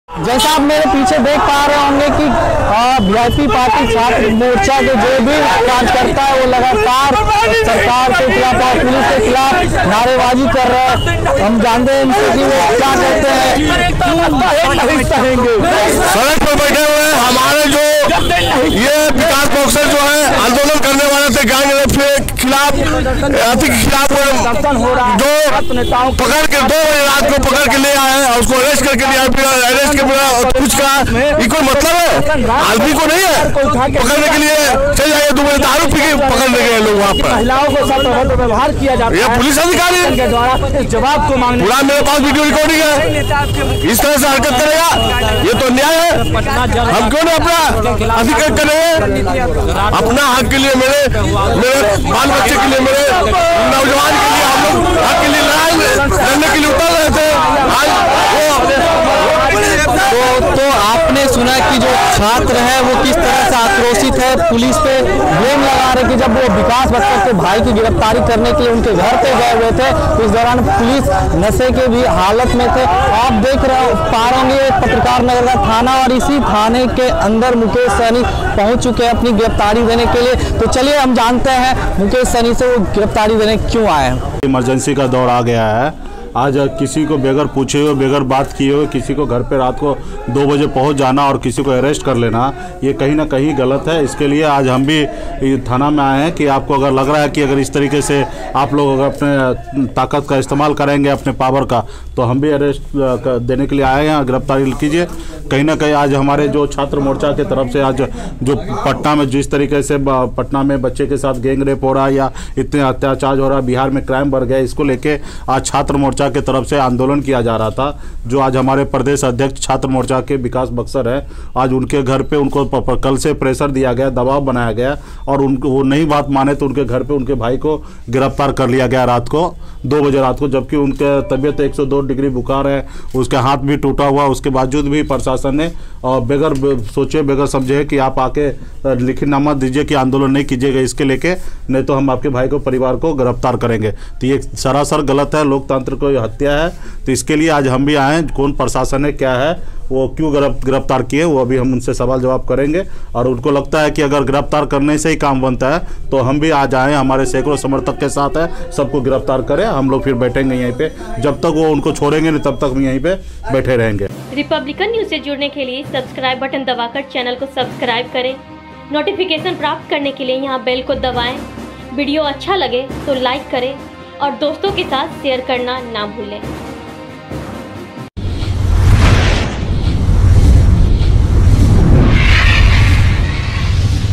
Patna के इस Police Station में तमाशा, Mukesh Sahani पहुंचे गिरफ्तारी देने, जमकर हंगामा
'सन ऑफ मल्लाह' मुकेश सहनी का हाई वोल्टेज ड्रामा देखिए। पुलिस थाने में पहुंच गए गिरफ्तारी देने। समर्थक खूब कर रहे हंगामा। गिरफ्तारी के पीछे की वजह भी जानिए।